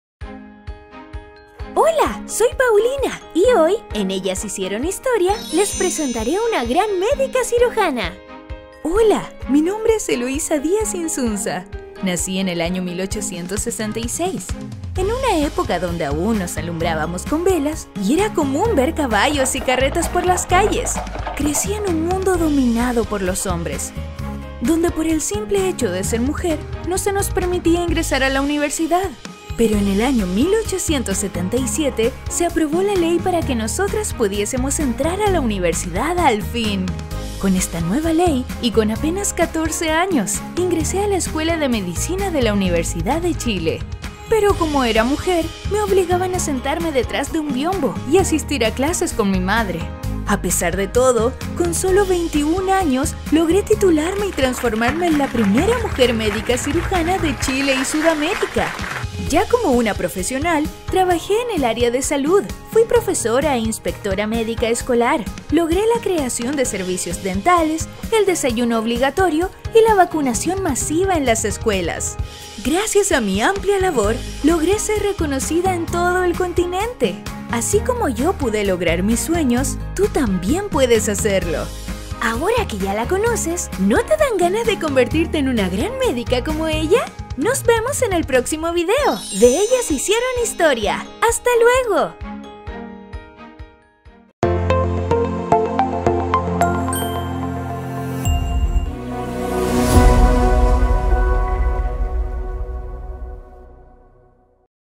Ellas hicieron historia. Capítulo 1
¿Sabías que muchas de las cosas que utilizas cotidianamente fueron inventadas por mujeres como tú? Aquí te presentamos a Eloísa Díaz Insunza, Primera médica cirujana de Chile y Latinoamérica 👩‍⚕️